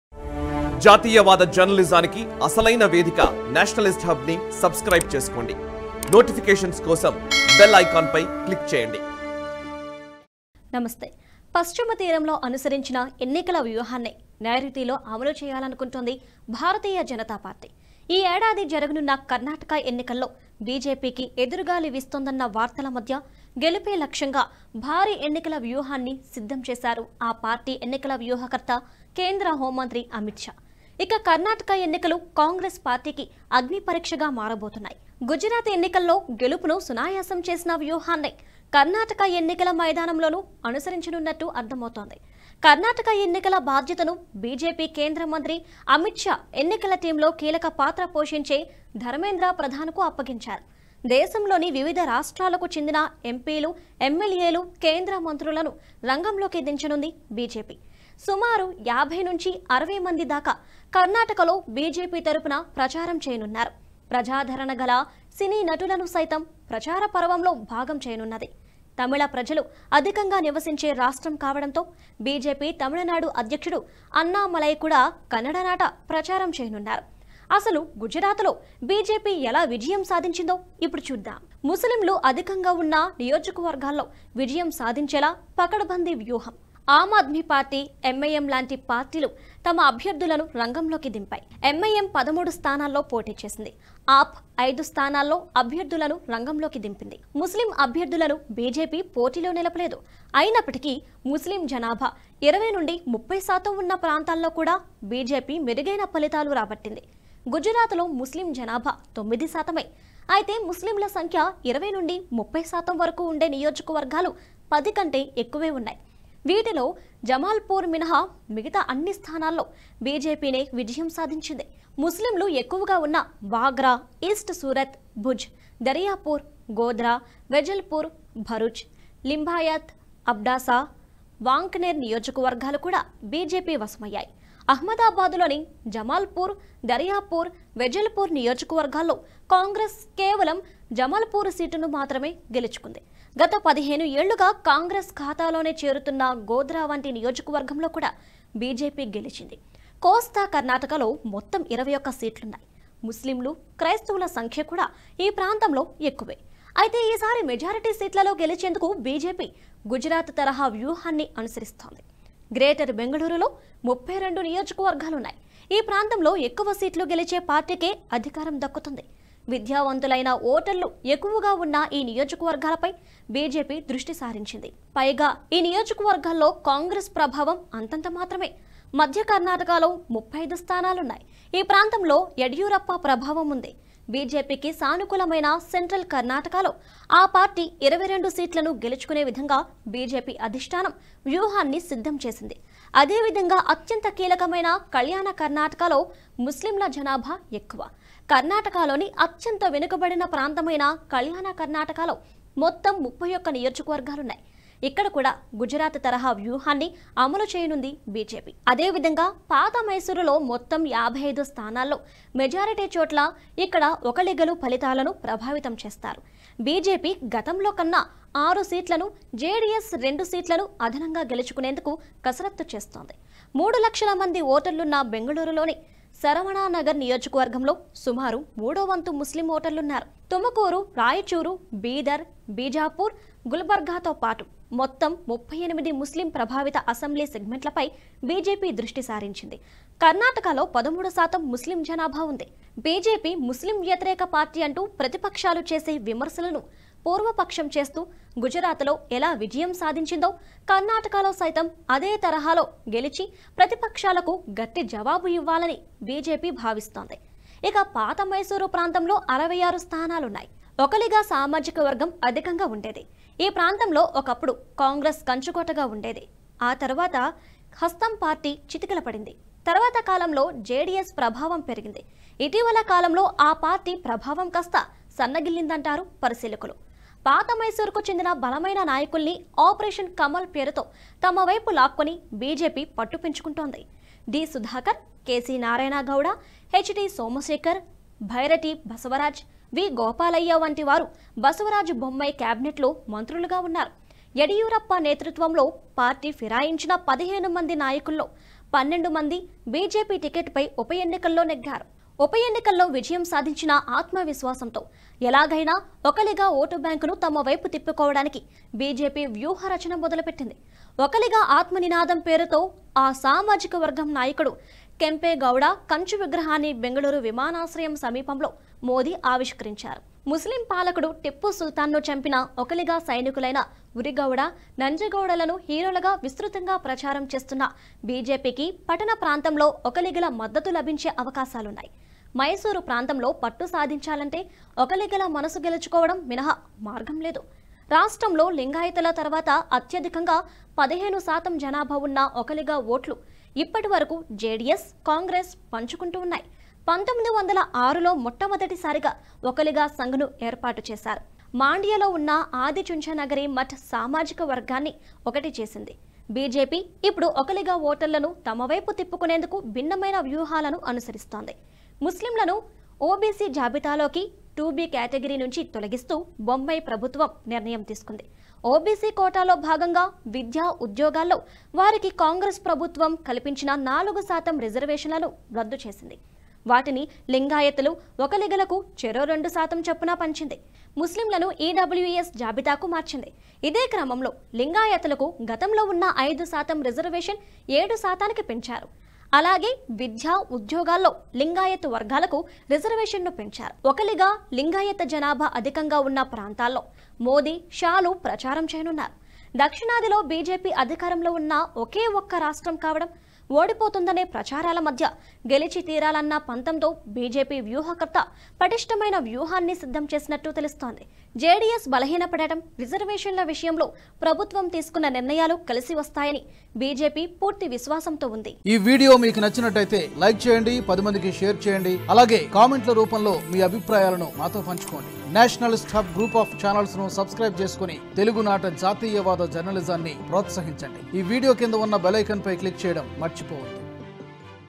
Special Story : Modi - Amit Shah Election Strategy In Karnataka Elections | Nationalist Hub
Our Company account details :
IFSC Code: HDFC0000512